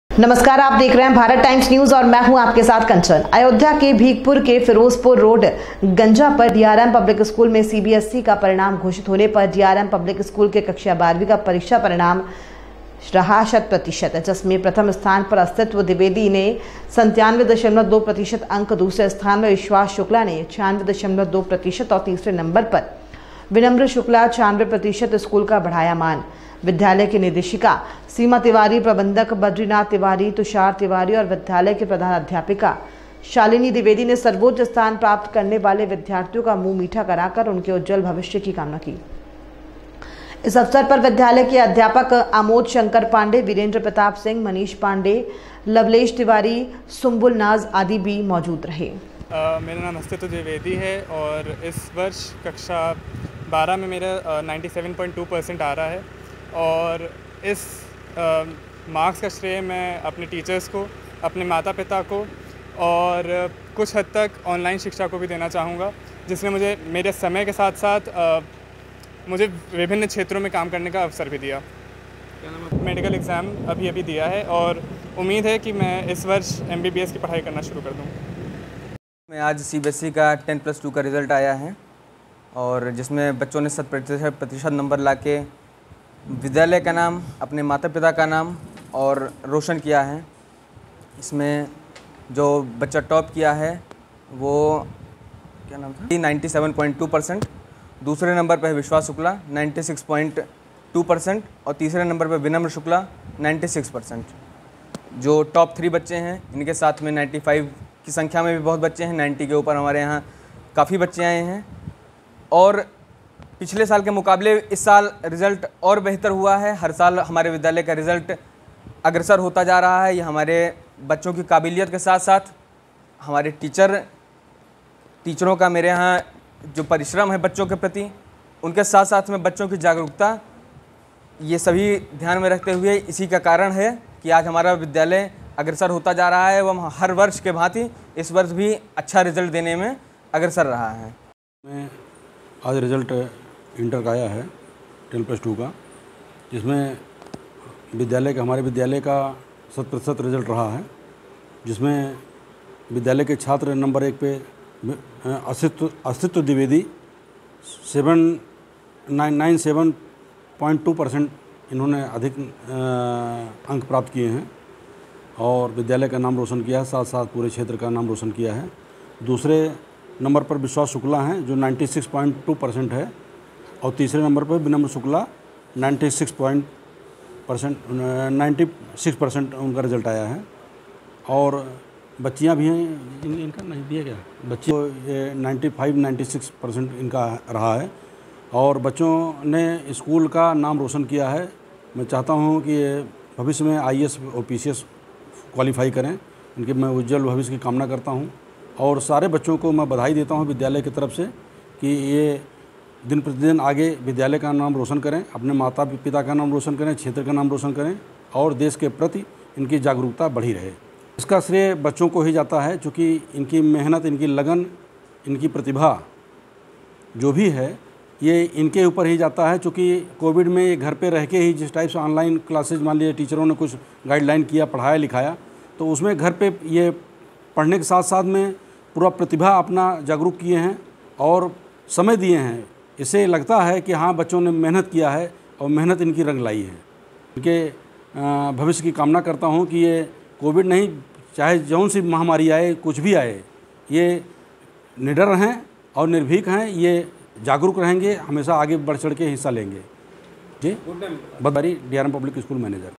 डी.आर.एम. पब्लिक स्कूल के अस्तित्व का द्विवेदी 97.2%
अयोध्या के भीखपुर के फिरोजपुर रोड गंजा पर डीआरएम पब्लिक स्कूल में सी.बी.एस.ई का परिणाम घोषित होने पर डी.आर.एम. पब्लिक स्कूल के कक्षा 12 का परीक्षा परिणाम रहा शत् प्रतिशत।जिसमे प्रथम स्थान पर अस्तित्व द्विवेदी ने 97.2% अंक, दूसरे स्थान में विश्वास शुक्ला ने 96.2% व तीसरे नंबर पर विनम्र शुक्ला 96% स्कूल का बढ़ाया मान।विद्यालय की निदेशिका सीमा तिवारी प्रबंधक बद्री नाथ तिवारी, तुषार तिवारी तथा विद्यालय की प्रधानाध्यापिका शालिनी द्विवेदी ने सर्वोच्च स्थान प्राप्त करने वाले विद्यार्थियो का मुंह मीठा कराकर उनके उज्ज्वल भविष्य की कामना की। इस अवसर पर विद्यालय के अध्यापक आमोद शंकर पाण्डे ,वीरेन्द्र प्रताप सिंह मनीष पाण्डे ,लवलेश तिवारी, सुम्बुल नाज. आदि भी रहे मौजूद।

Byte- बद्री नाथ तिवारी प्रबंधक डी0आर0एम0 पब्लिक स्कूल।

Byte- तुषार तिवारी मैनेजिंग डायरेक्टर डीआरएम पब्लिक स्कूल।

Byte- अस्तित्व द्विवेदी टॉपर डीआरएम पब्लिक स्कूल।